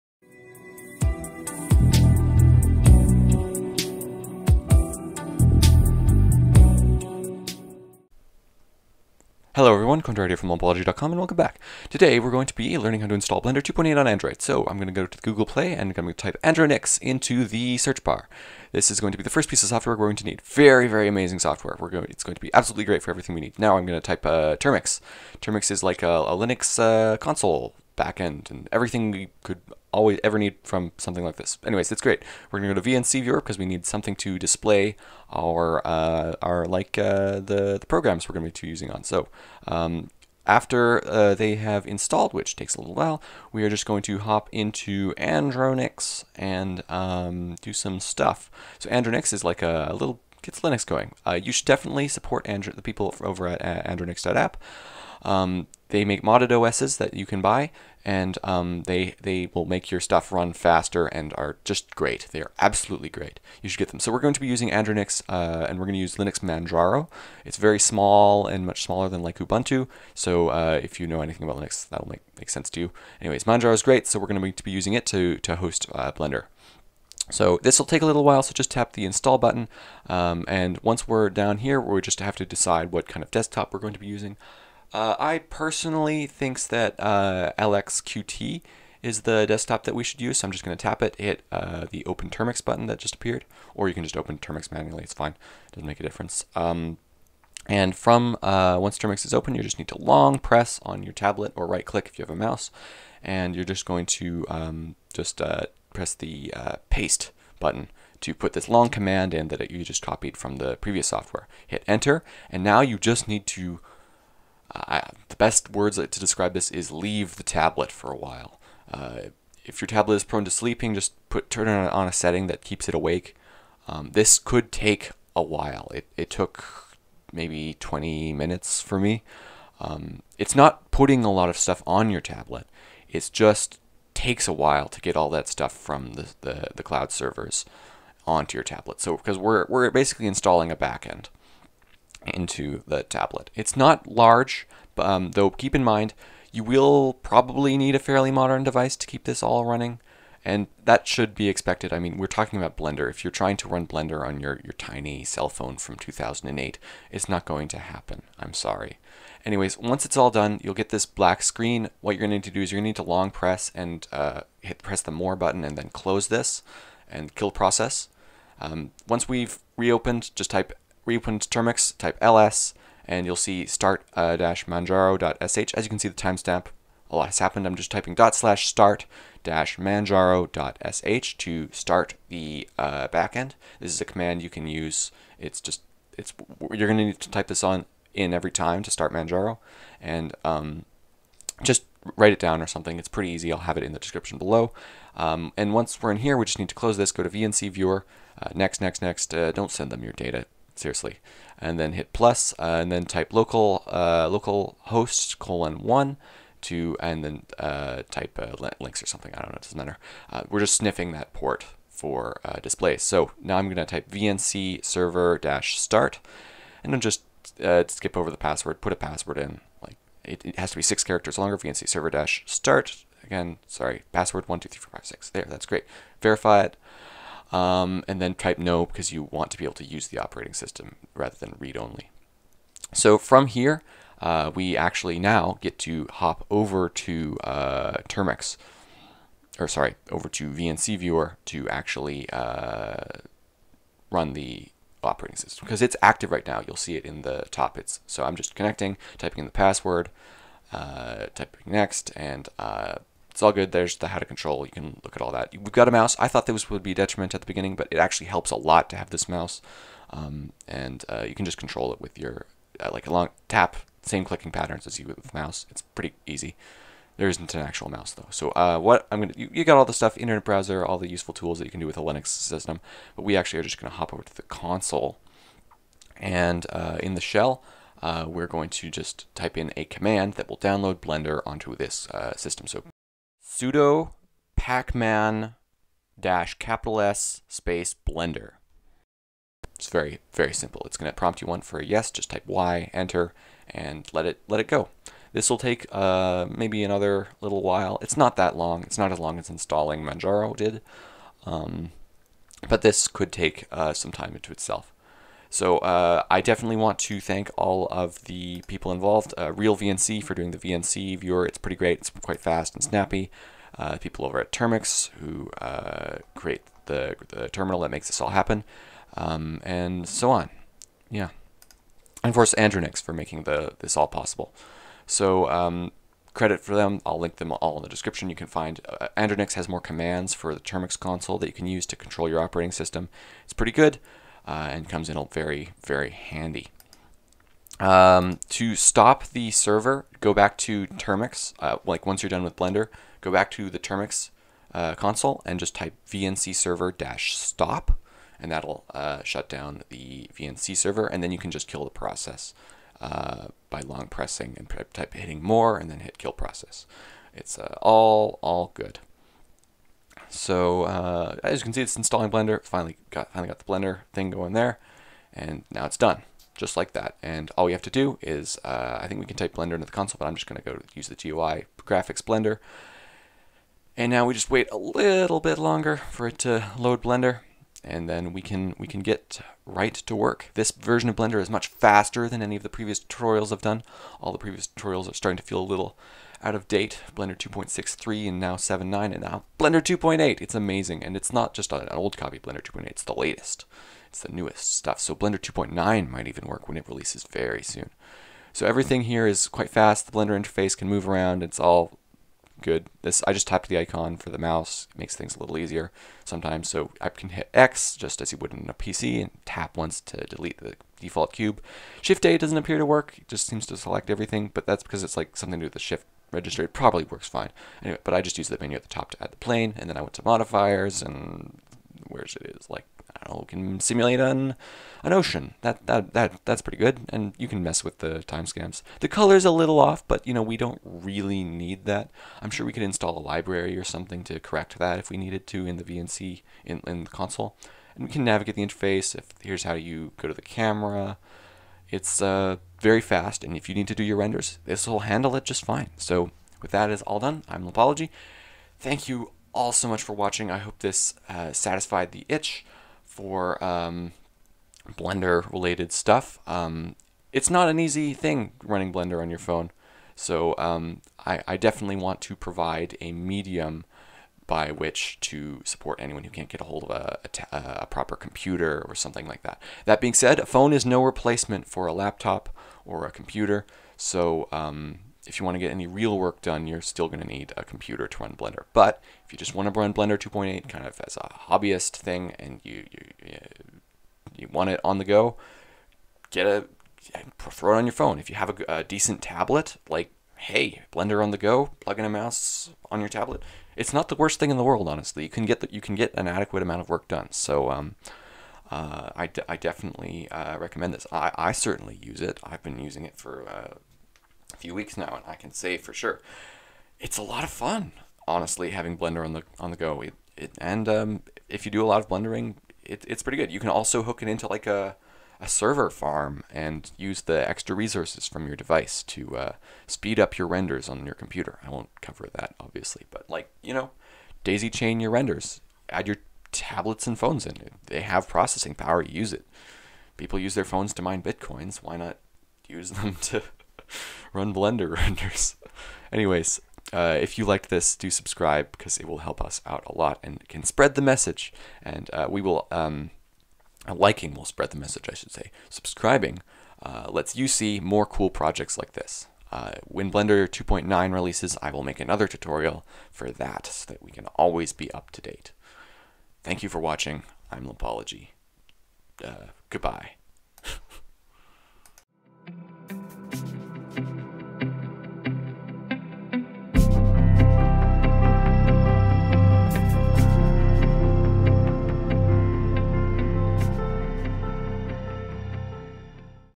How To Install Blender 2.8 On Android
This tutorial is a big improvement on the last two, hope this does a better job!

Any questions or support requests are recommended to be posted in one of these two locations:

Open Andronix and install the Manjaro Linux distro using Termux.

VNC Viewer needs to sniff: localhost:1

Commands:
ls
./start-manjaro.sh
vncserver-start

sudo pacman -S blender

vncserver-stop